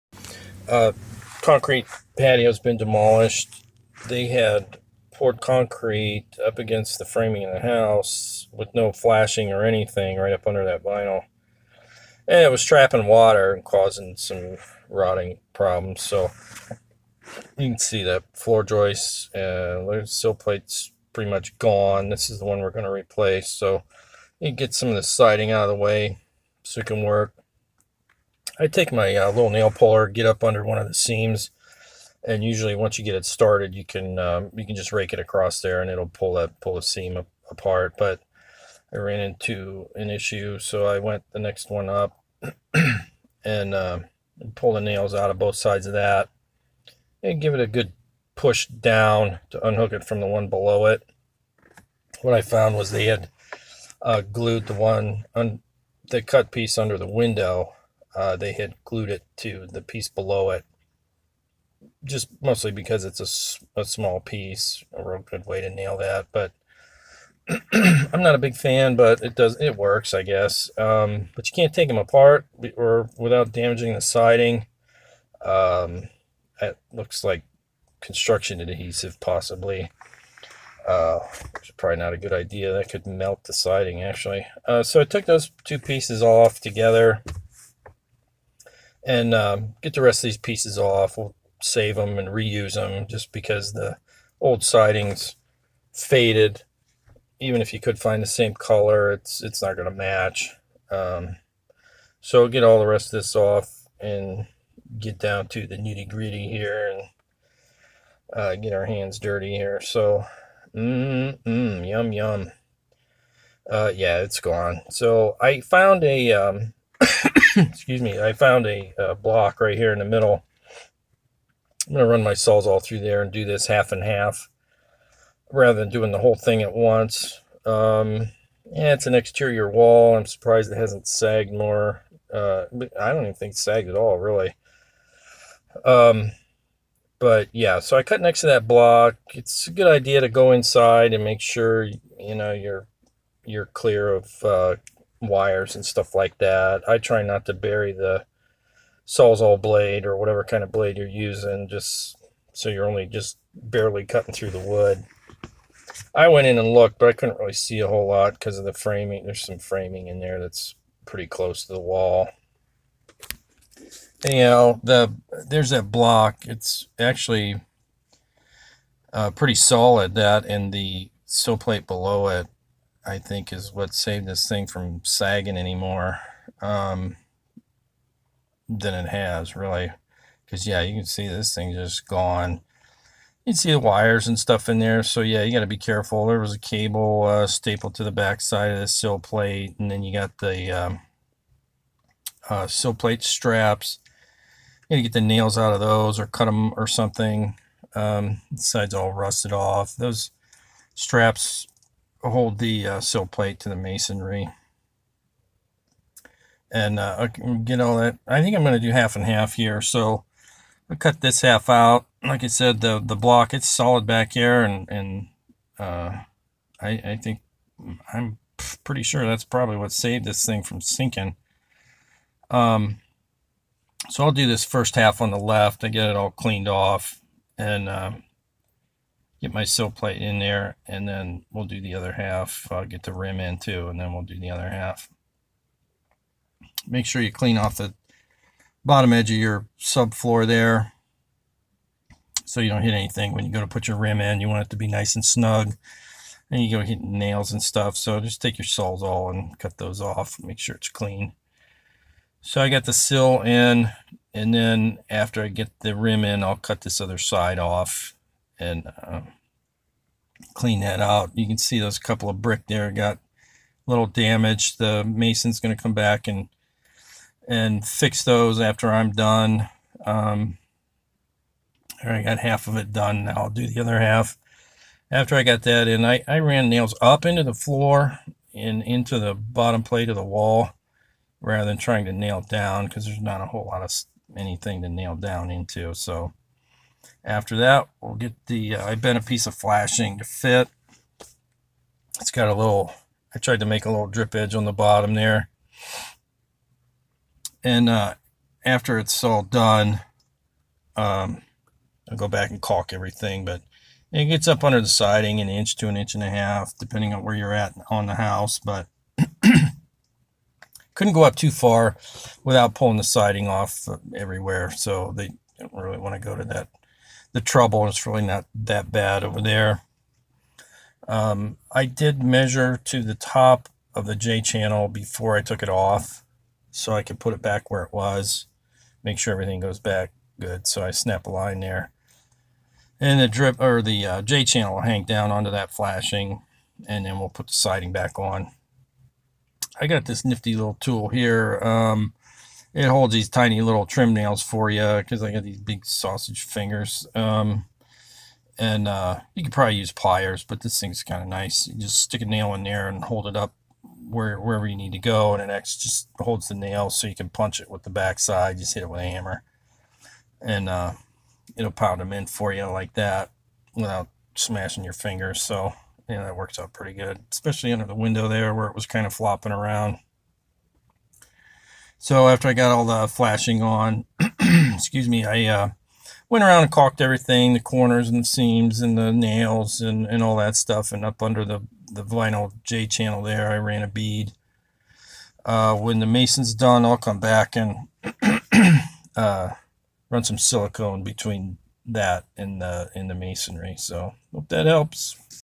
how to replace a rotten rim joist
Tips on removing/replacing rotten rim joist. Remove /replace vinyl siding and install new flashing. (Note: Code doesn't require pressure treated sill plate as long as there's some sort of sill seal barrier between sill and masonry, which there is)